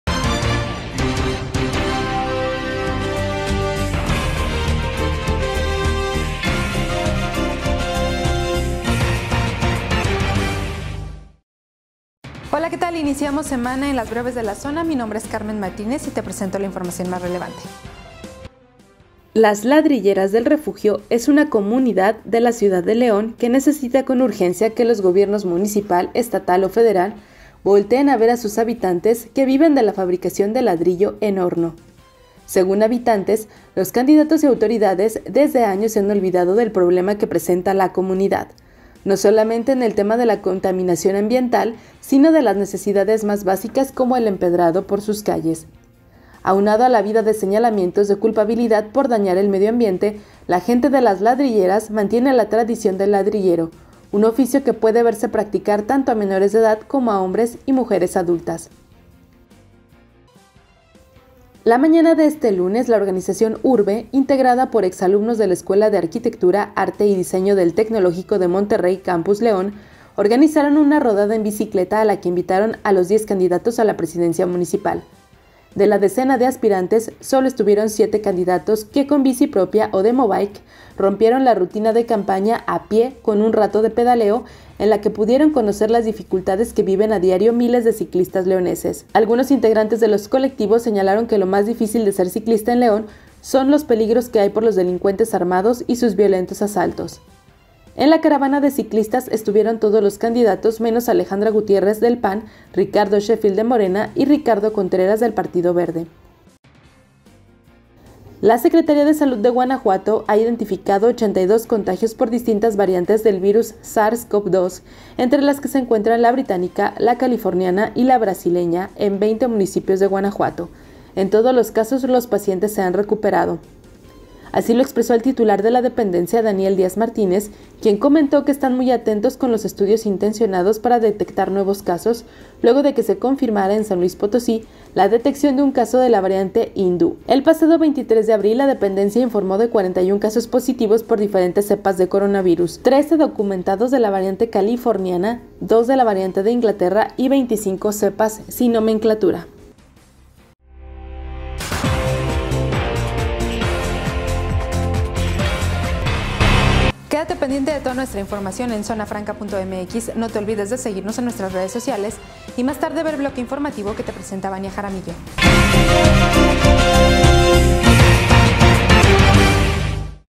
Suma Guanajuato 82 casos de variantes de COVID-19 | BrevesDeLaZona📰
Estas son las ÚltimasNoticias en Guanajuato
- Así es la vida de los habitantes de las Ladrilleras del Refugio, en León.
- Se suman candidatos a la alcaldía de León a las necesidades de los ciclistas.
- Suma Guanajuato 82 casos de variantes de COVID-19; todos se han recuperado.